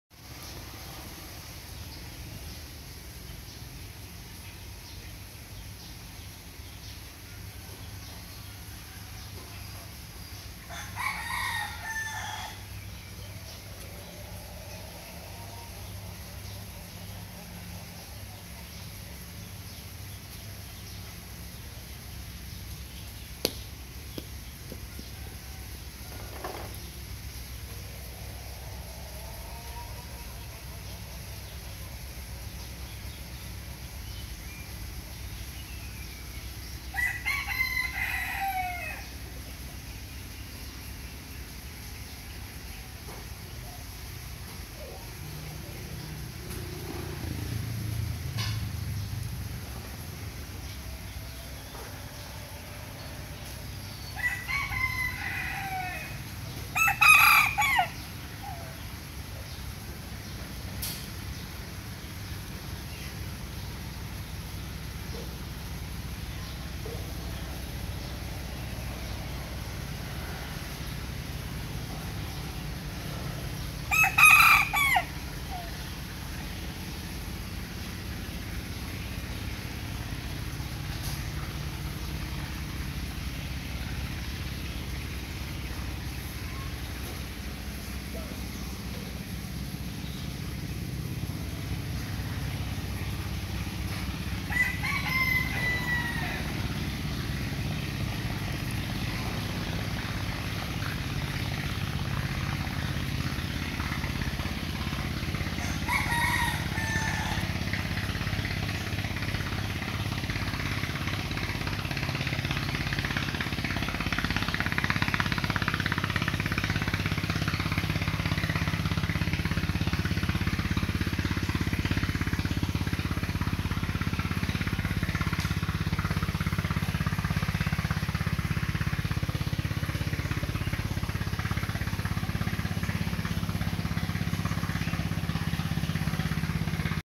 Hai Con Gà Tre Tắm Cát Trong Chậu Bông
Tôi được quay hai con gà tre đang tắm cát trong một chậu bông.